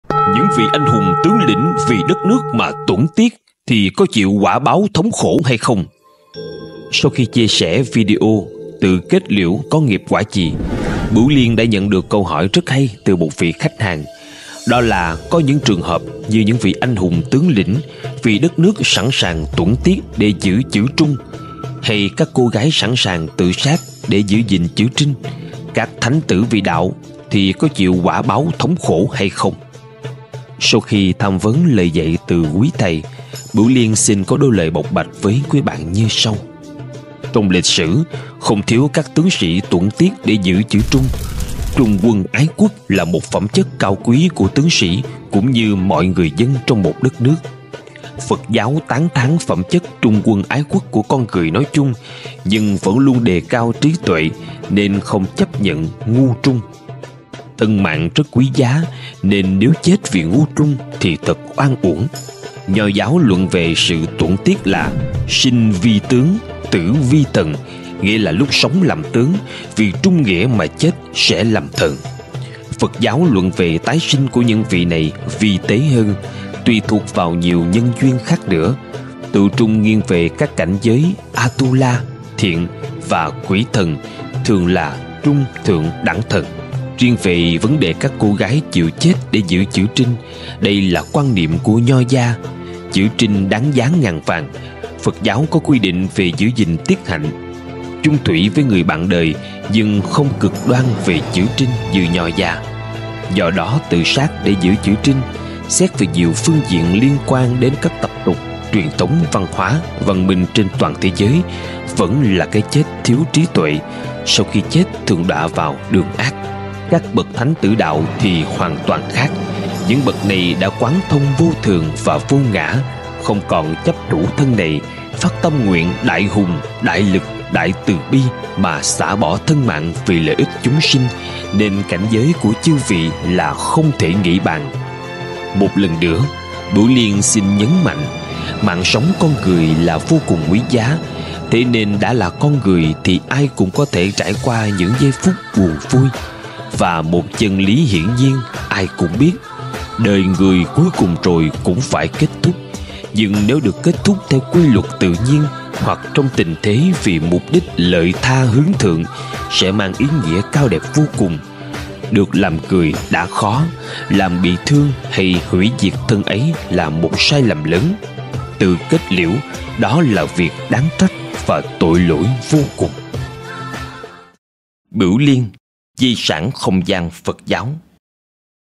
Những vị anh hùng vì đất nước mà tuẫn tiết
Những vị anh hùng tướng lĩnh vì đất nước mà tuẫn tiết thì có chịu quả báo thống khổ hay không?